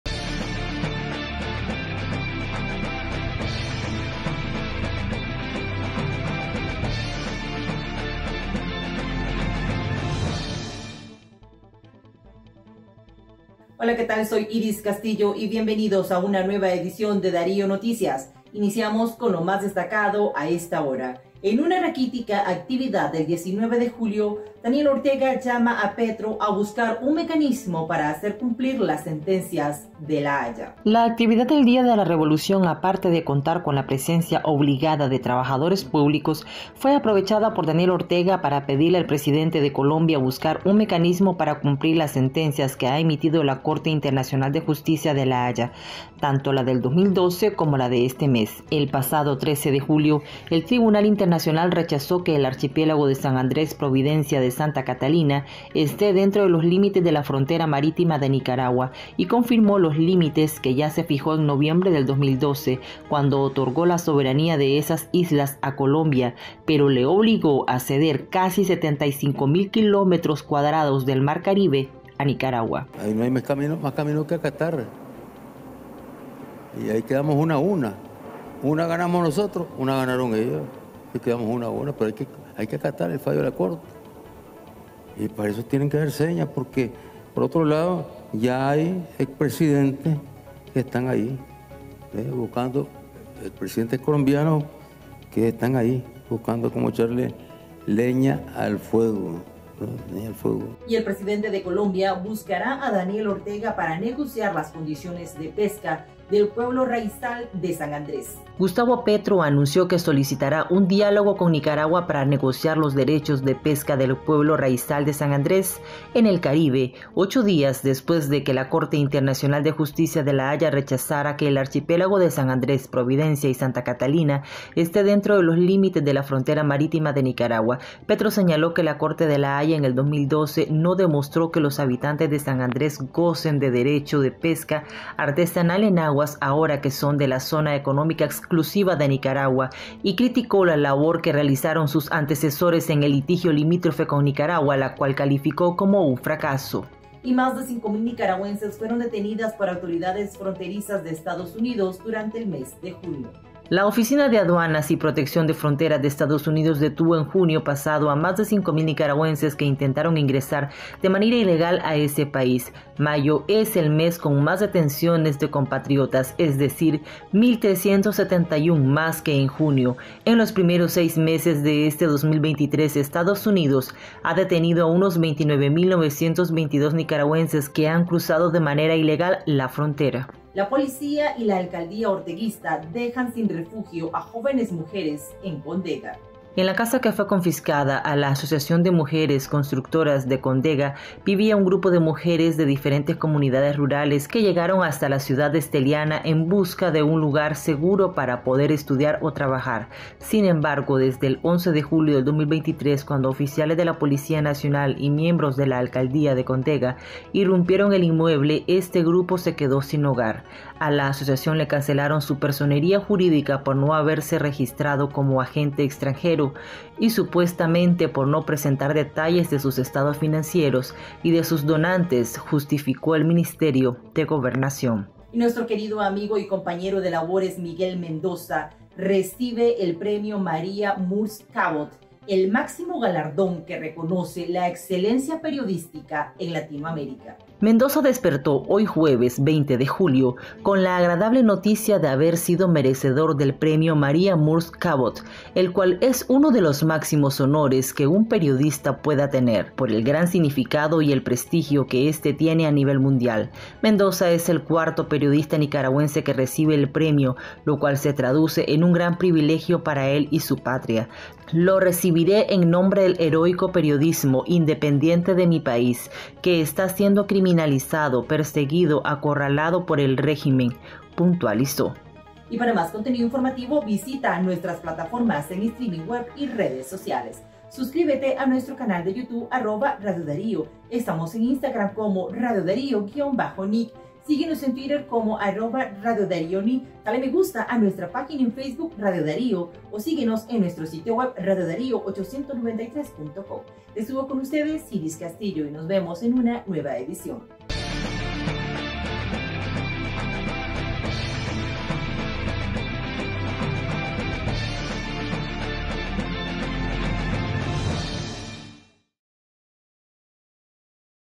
Ortega llamó a Petro a buscar un mecanismo para cumplir las sentencias de La Haya
En raquítica actividad del 19 de julio, Ortega llamó a Petro a buscar un mecanismo para cumplir las sentencias de La Haya. Esto y más en el noticiero de RadioDarioNi